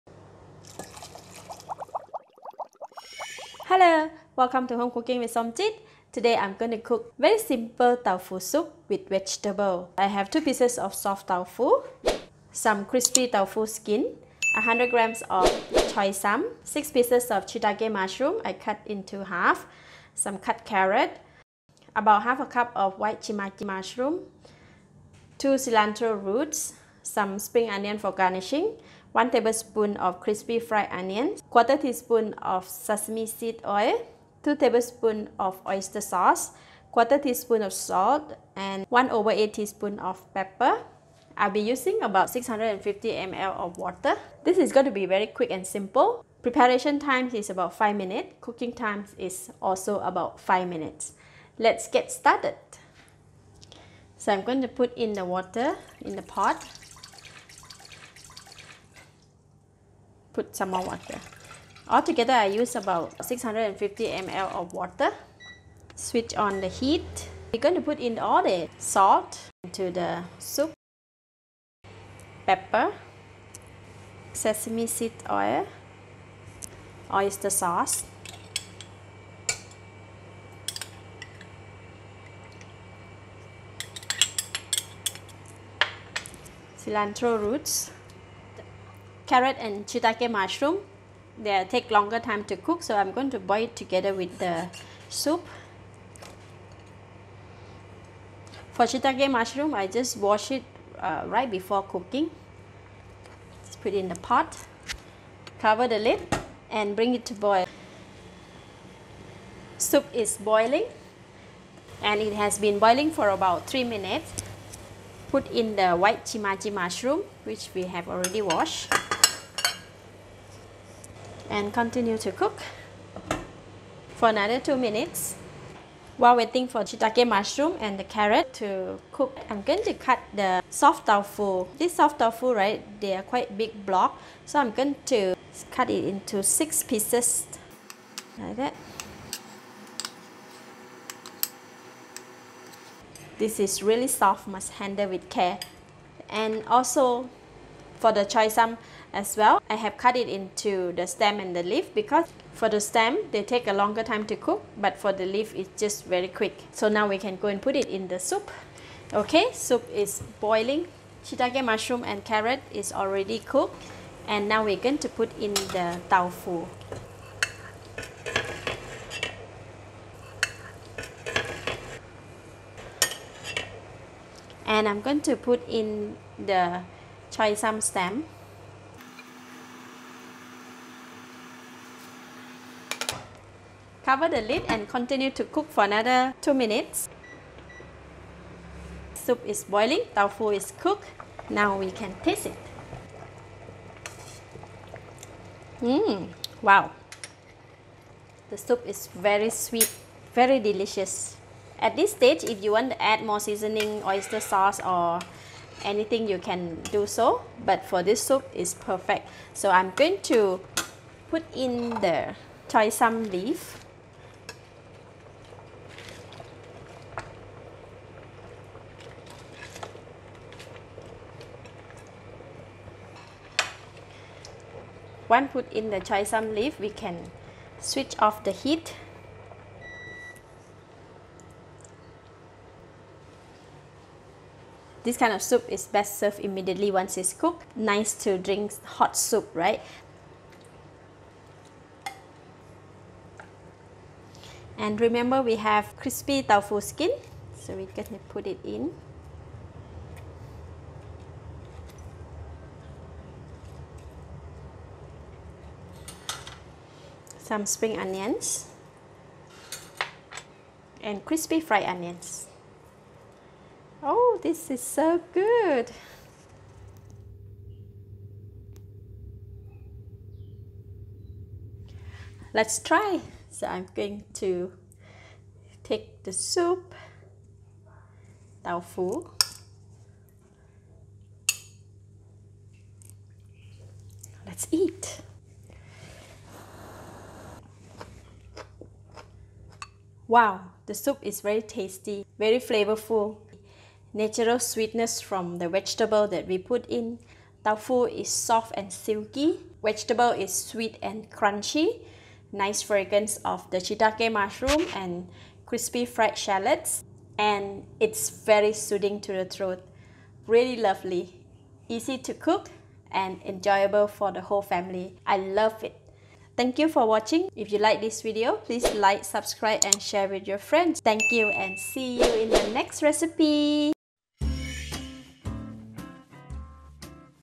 Simple Tofu Soup with Vegetables (No Meat Recipe)
Learn how to make Tofu Soup that is simple, packed with nutrients and delicious, without using meat! This recipe is perfect for fast meal for the family. Turn it into a vegetarian recipe by substituting Oyster Sauce with Mushroom Sauce! Ingredients & Utensils ↓

Serves 4.
Store not more than 1 day in the fridge.

Ingredients:
2 pieces Soft Tofu (from wet market or this brand works)
650ml Water